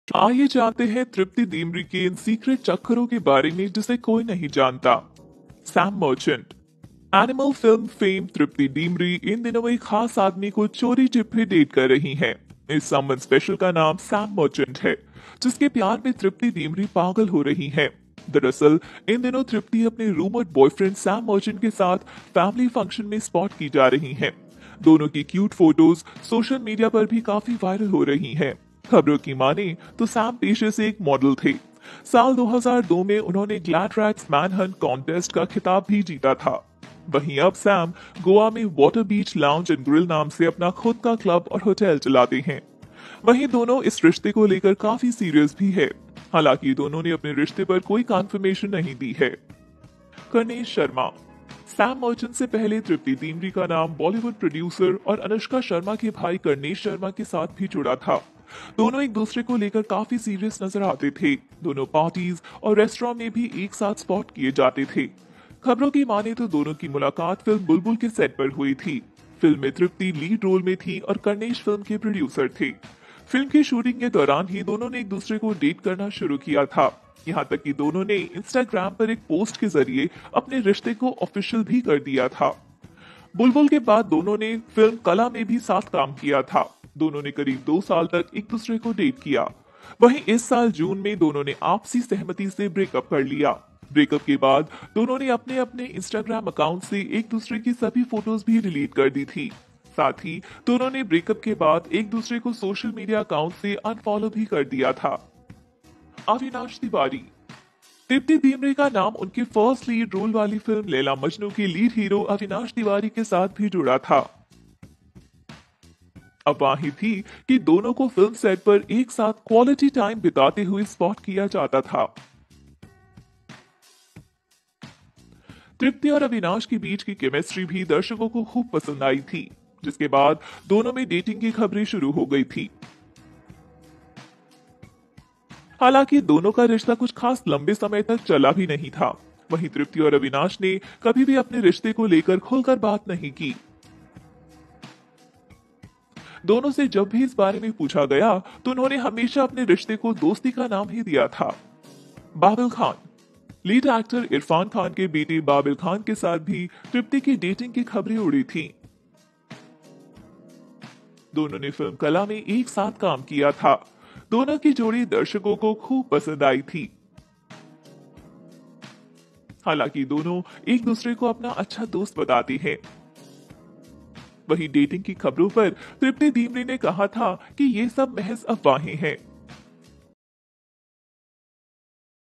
Bollywod Actores तृप्ति डिमरी का सीक्रेट चक्कर !
Bollywood actress तृप्ति डिमरी का सीक्रेट चक्कर !

नेशनल क्रश तृप्ति डिमरी इस शख्स के साथ आईं नजर तो फैन्स को आ गया गुस्सा, बोले- भाभी से दूर हो

अब इन दिनों तृप्ति डिमरी की तस्वीरें सोशल मीडिया सेलिब्रिटी ओरी उर्फ ओरहान अवत्रमणि के साथ वायरल हो रही है. जिसे देख एक्ट्रेस के फैंस सहित तमाम सोशल मीडिया पर यूजर रिएक्शन दे रहे हैं.